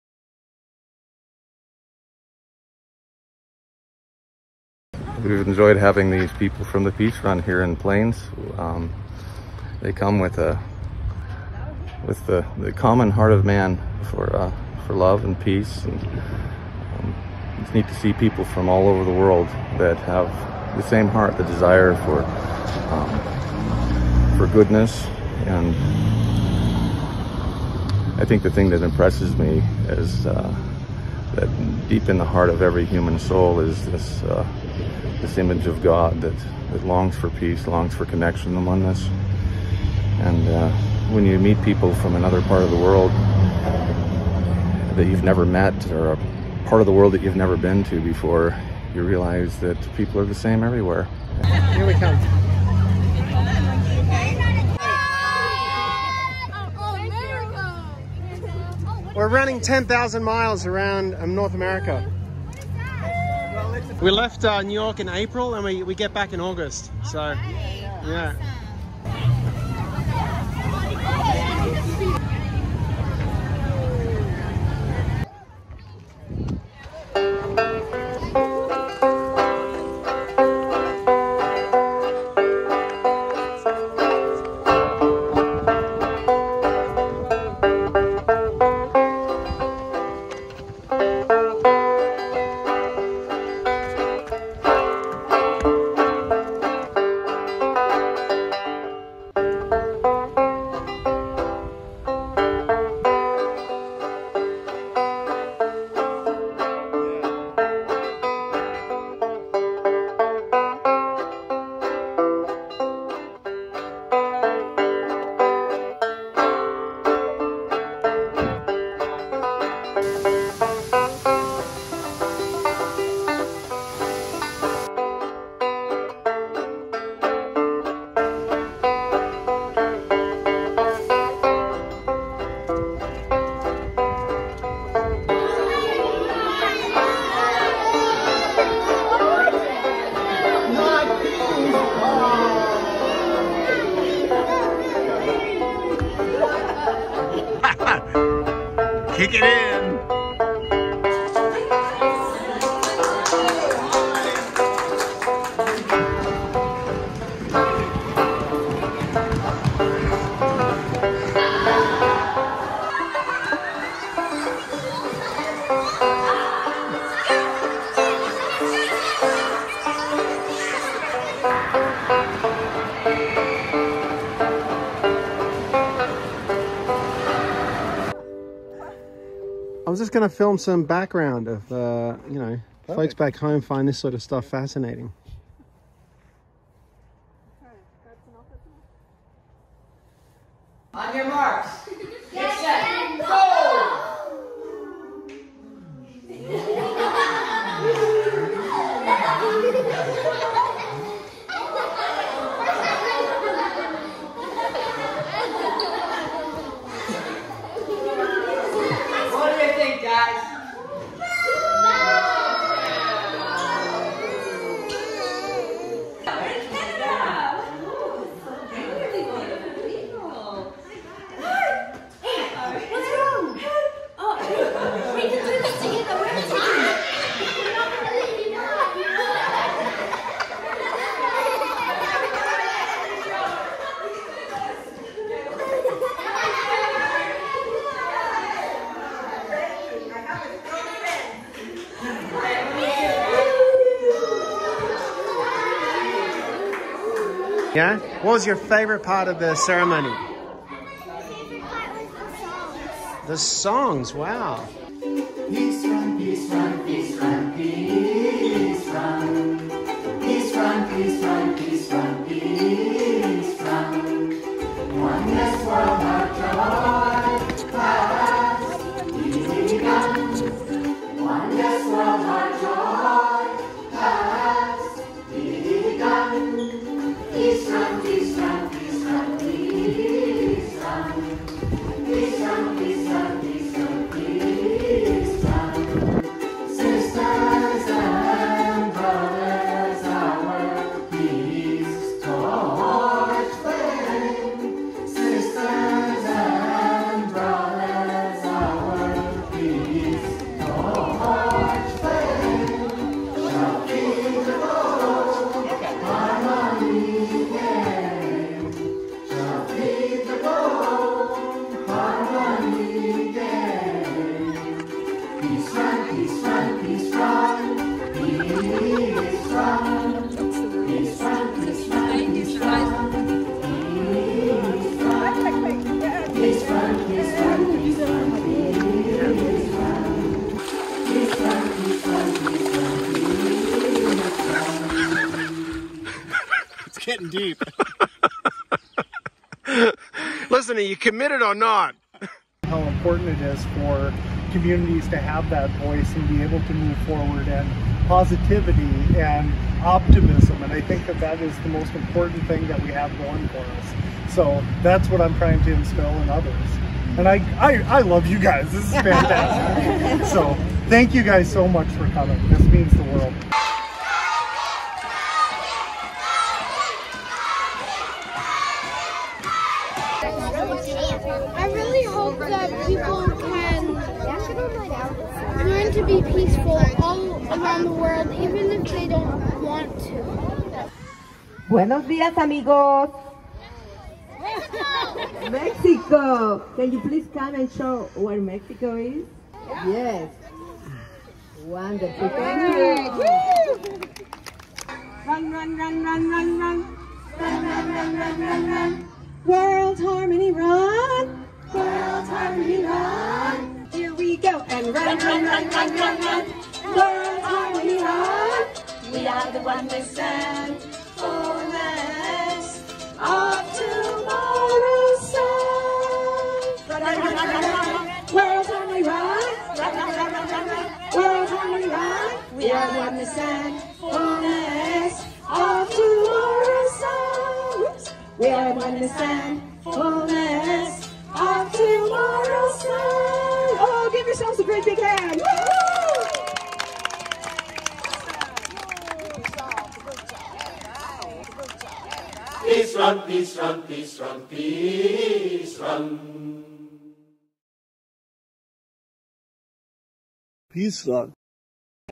Northwest to Canada to Minneapolis, MN - Peace Run 2024
Follow the team as they make their way through Big Sky Country into the Canadian Prairies and arriving into Minneapolis, Minnesota. This video is guaranteed to bring a smile to your face. Enjoy.